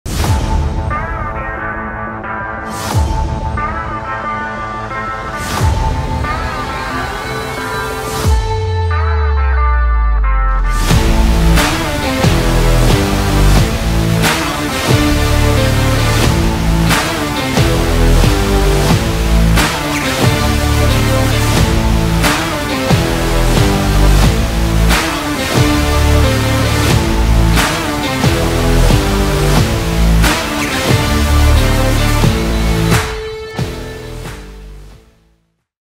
Bear Creek Arsenal GPX Trailer: Coming...
Get hyped for the all new Bear Creek Arsenal GPX gas piston AR-15, coming April 11, 2022.

Find the best AR-15, AR-10, and AR-9 rifles, pistols, and parts at Bearcreekarsenal.com!